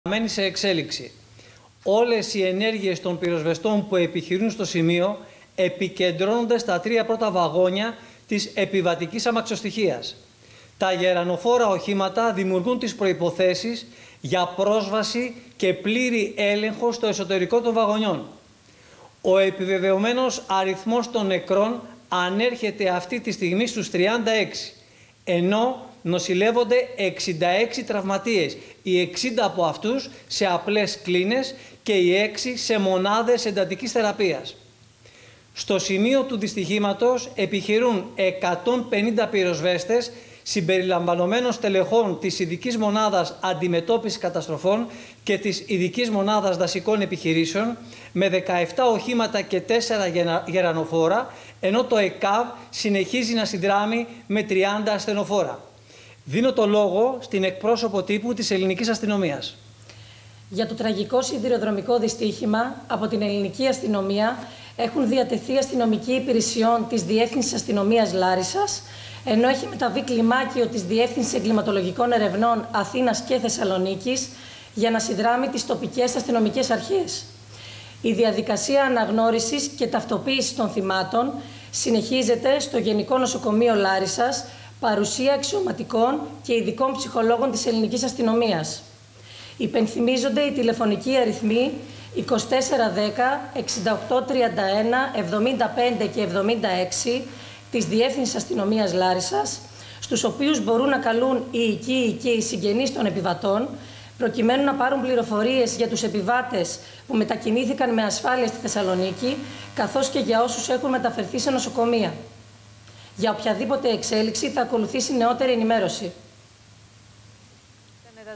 Πολύνεκρη σύγκρουση τρένων στα Τέμπη - Νέες δηλώσεις εκπρόσωπος Τύπου Πυροσβεστικής και Αστυνομίας
38 είναι οι νεκροί μέχρι τώρα από την πολύνεκρη σύγκρουση τρένων στα Τέμπη.

Οι τραυματίες είναι 130, από αυτούς οι 66 νοσηλεύονται σε νοσοκομεία της Λάρισας, της Κατερίνης και της Θεσσαλονίκης.

Συνεχίζουν να ψάχνουν επιβάτες και εργαζόμενους σε σωρούς μετάλλων που ήταν βαγόνια, οι πυροσβέστες.

Ωστόσο εκφράζονται φόβοι ότι είναι εξαιρετικά δύσκολο να εντοπισθούν επιζώντες στα πρώτα βαγόνια που σχεδόν διαλύθηκαν...

Στα τηλέφωνα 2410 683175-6 μπορούν να τηλεφωνούν οι συγγενείς επιβατών για να ενημερωθούν για τους ανθρώπους τους.